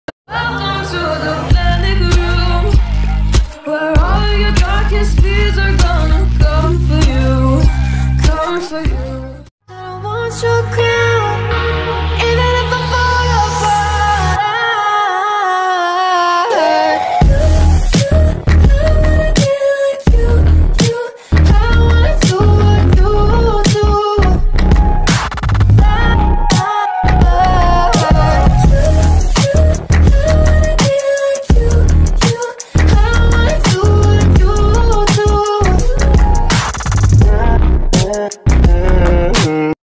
|l be like you meme l| ft. My fav gachatubers
Hi!~

The gachatubers in this video (in order starting from my most fav)
1. Shadow kun
2. Gacha nosa
3. Phoenix Gacha
4. Itz angiiee

Hope you guys have a nice day!